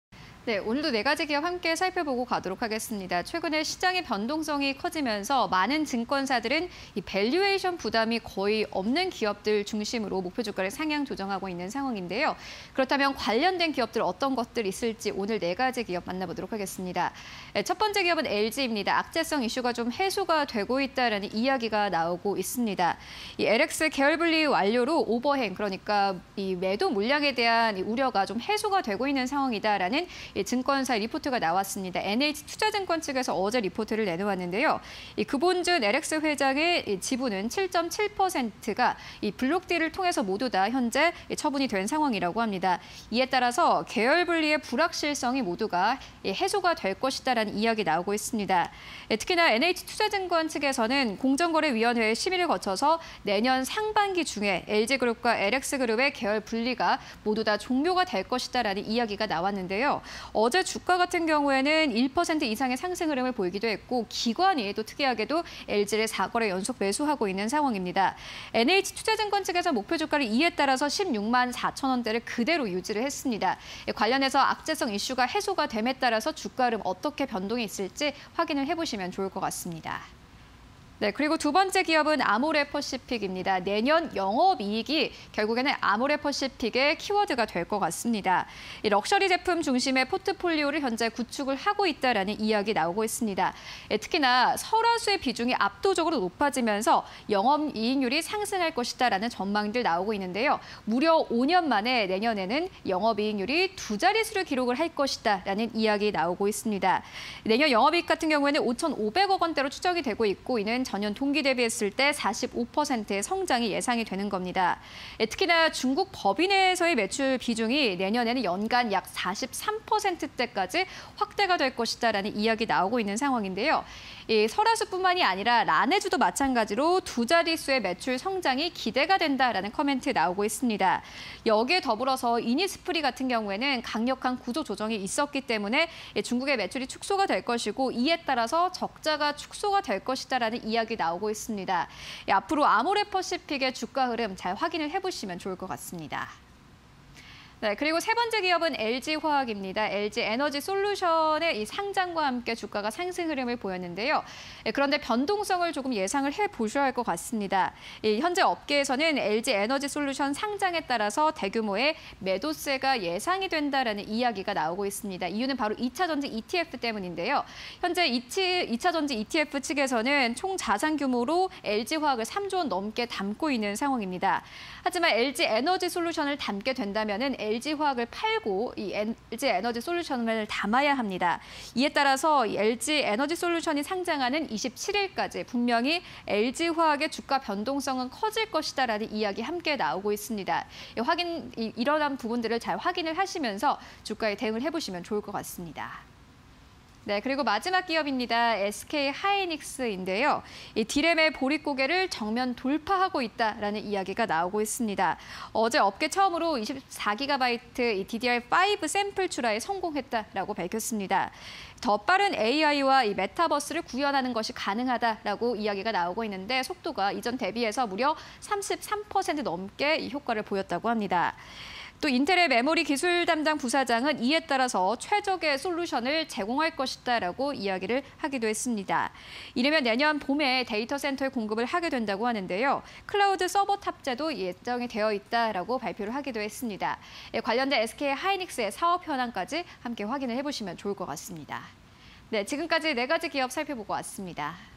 LG, 아모레퍼시픽, LG화학, SK하이닉스_공시&뉴스 (20211216)
- LX 계열분리 완료로 오버행 우려 해소
- 주식 교환으로 계열분리 불확실성 해소
- NH證 “목표주가 164,000원 유지… 투자의견 ‘매수’”

- 럭셔리 제품 중심 포트폴리오 구축
- 내년 영업이익 5,513억 원 추정… 45% 성장 예상

- LG에너지솔루션 상장으로 대규모 매도세 예상
- 2차전지 ETF 보유 중인 LG화학 매도 전망
- LG에너지솔루션, 27일 상장 예정… 이전까지 주가 변동성↑

- D램 시장 초격차 가속화… 업계 첫 24Gb DDR5 샘플 출하
- 이르면 내년 봄 데이터센터에 공급
- 클라우드 서버 탑재 예정